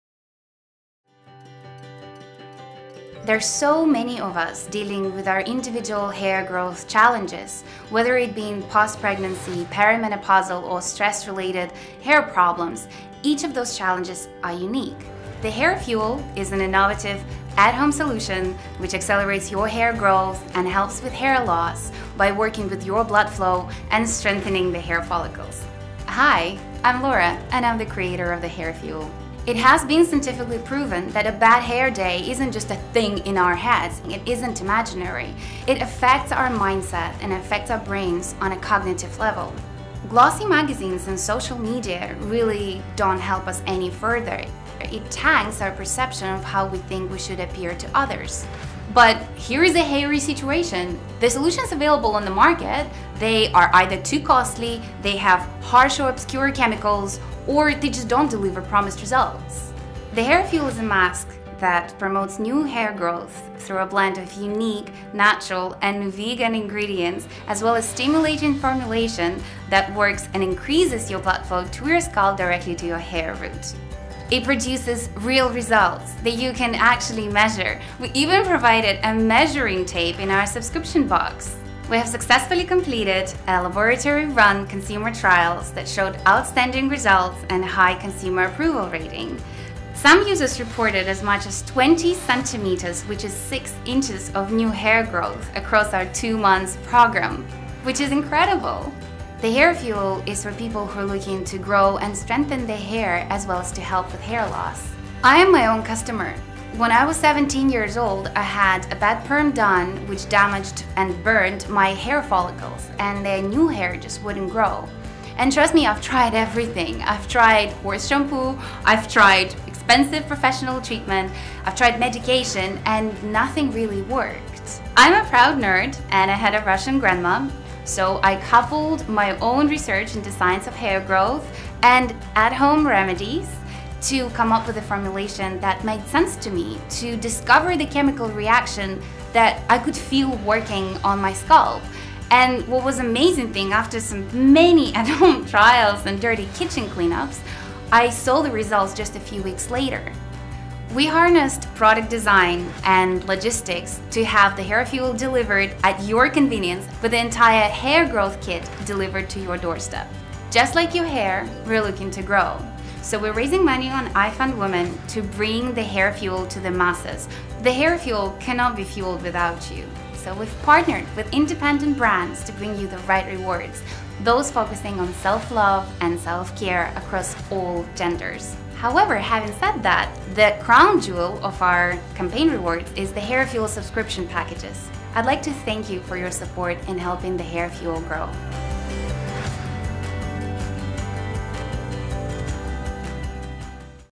The Hair Fuel – Hair Growth Mask – Crowdfunding Video
The Hair Fuel is an all natural, cruelty-free, vegan, paraben-, silicon- and sulfate-free hair growth mask that powers your hair growth from your roots. Our crowdfunding campaign has successfully reached our goal. You can order The Hair Fuel NOW

Your hair growth starts from where the hair grows: its roots and follicles. The Hair Fuel works directly with your blood flow by improving it as well as delivering nutrients from our unique mask blend directly to your root.